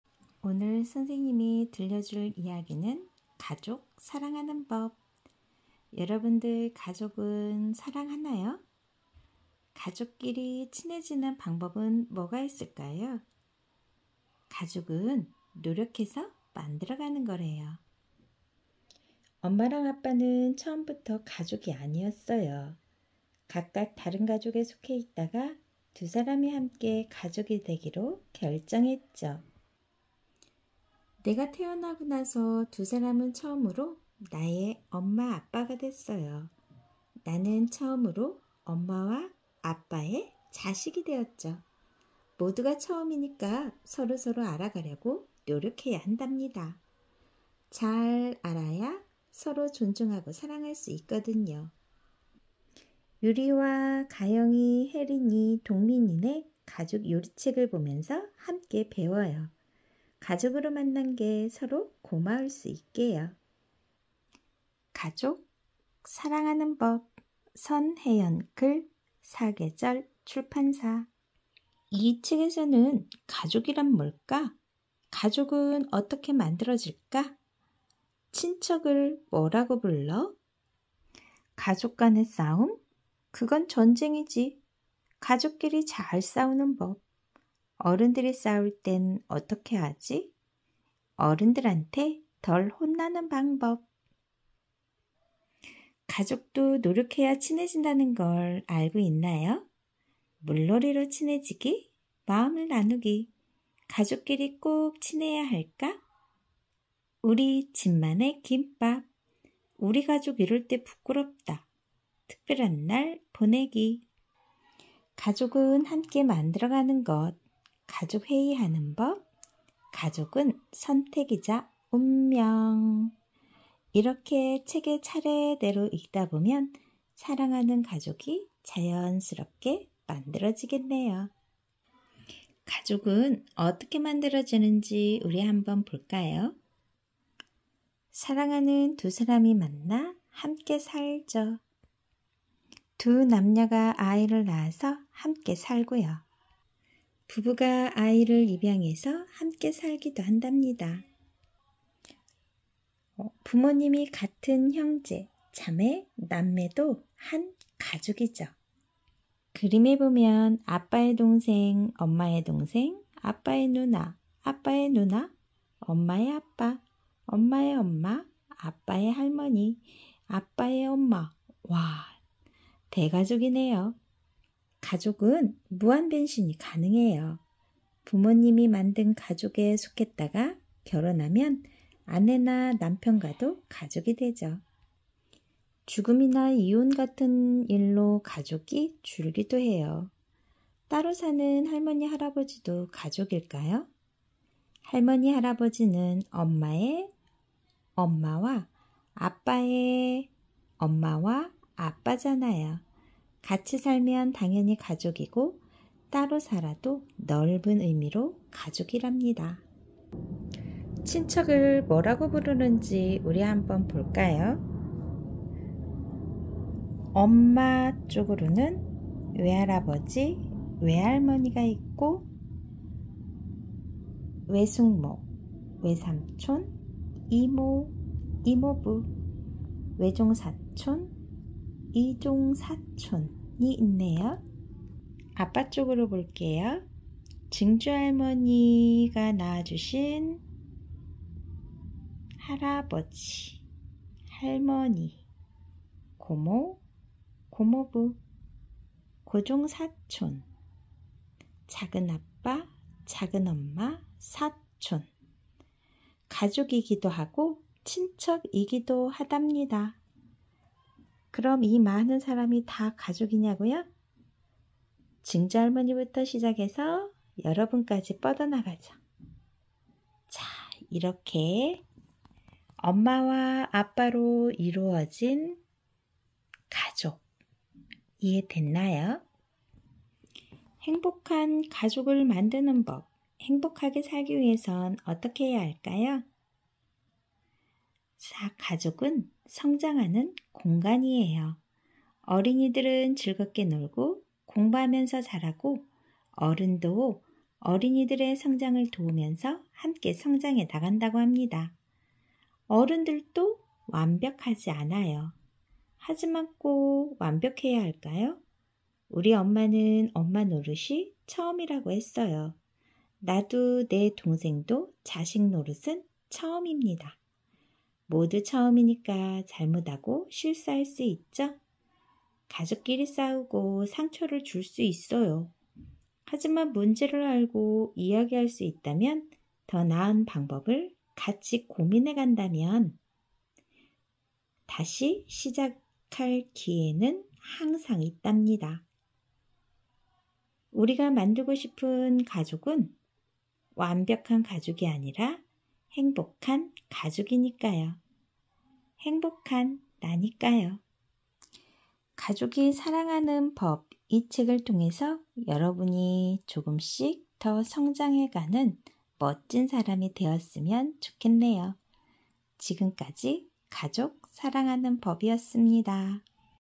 가족이 사랑하는법
가족이 되기 위한 첫 걸음
가족이 행복해 지기위해
어떻게 해야할지
생각해 보게 하는 책